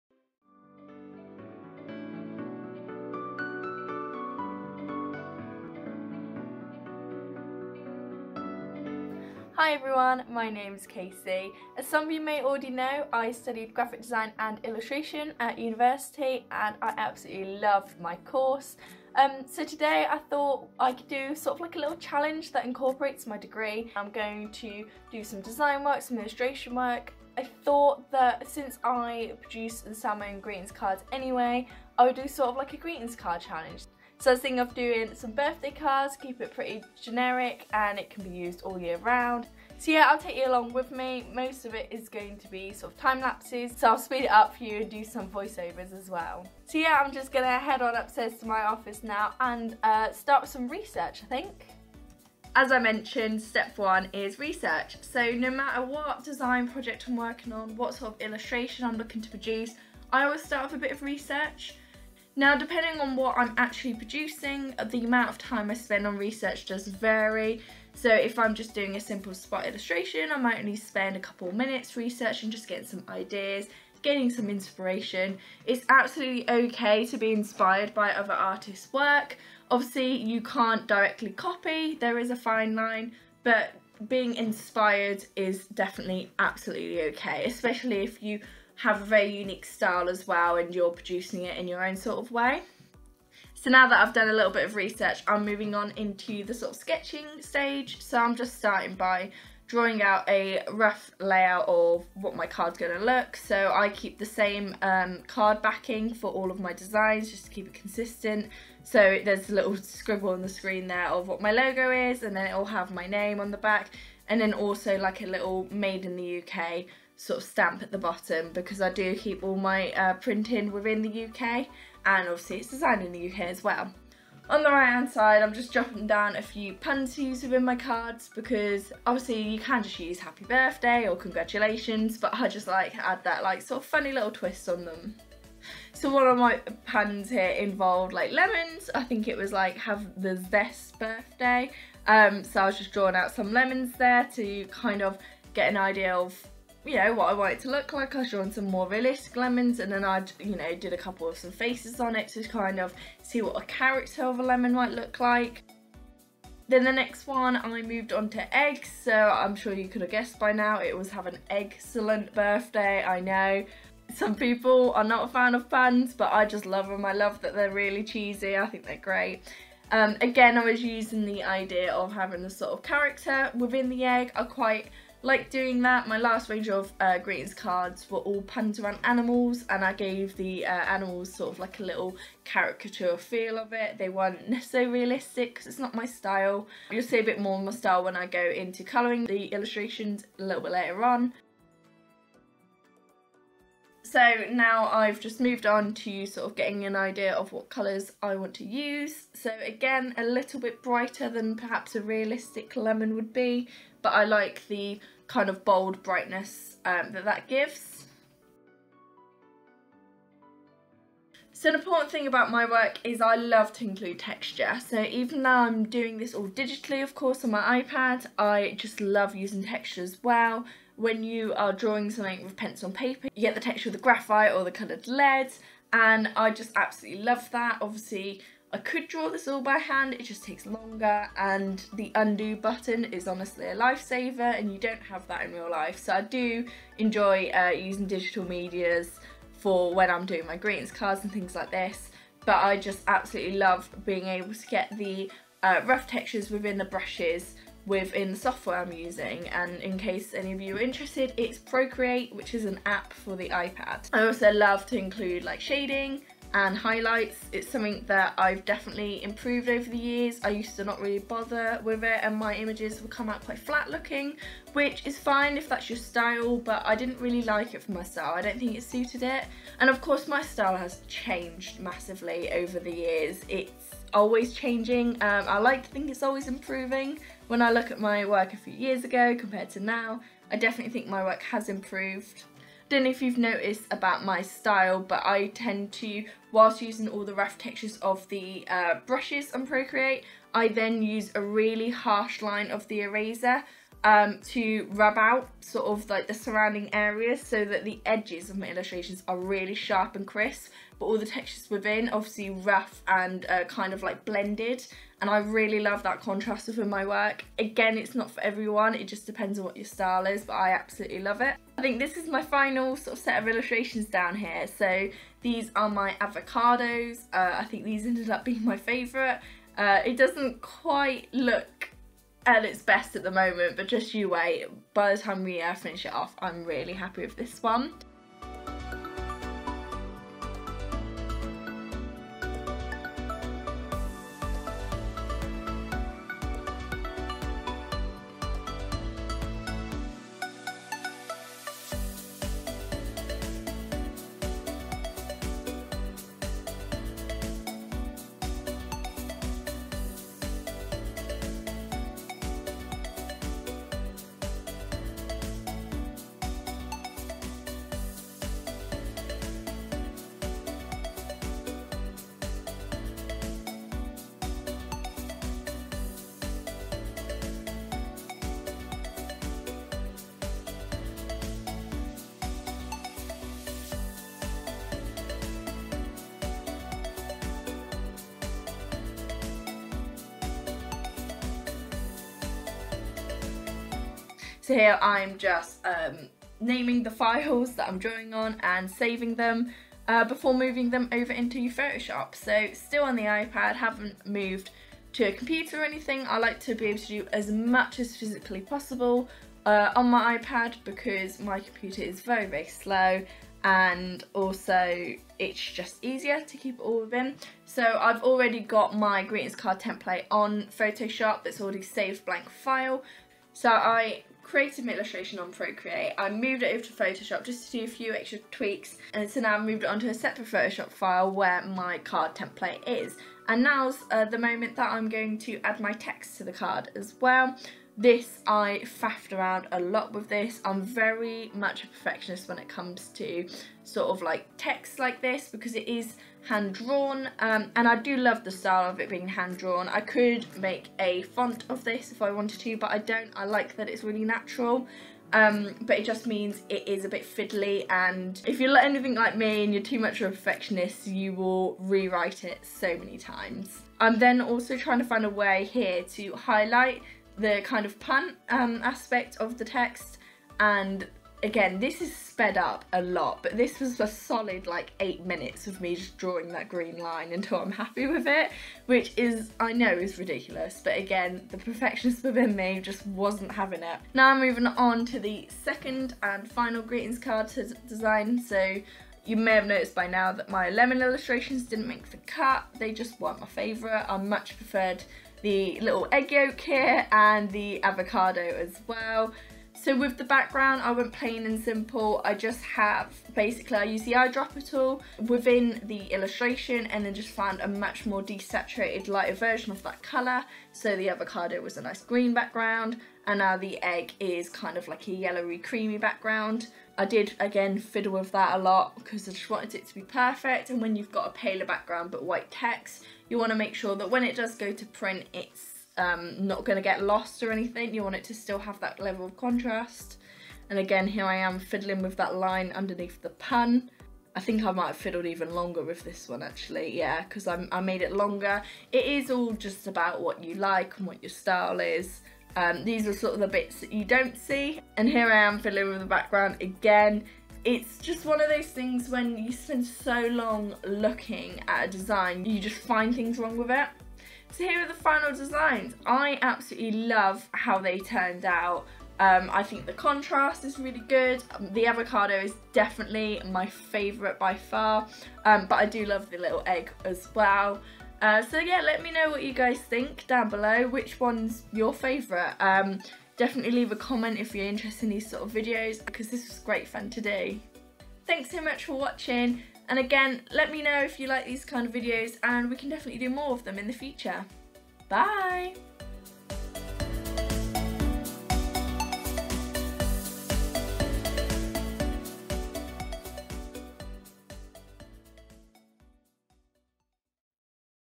Create my greetings cards with me | Pro Create Tutorial
Hi Everyone! Casey here, I thought I would show you my process for designing my greetings cards for my small business.

Let me know what you think and if you'd like to see more videos like this!

University Insiders Social media;